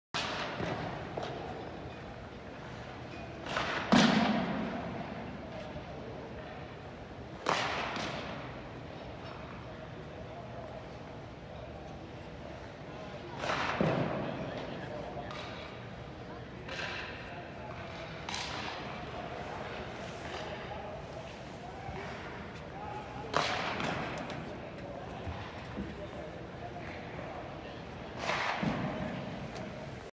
Predators Freddy Gaudreau, Matt Irwin practice Friday in Calgary
Healthy scratches Frederick Gaudreau, Matt Irwin during a shooting drill Friday after the team's morning skate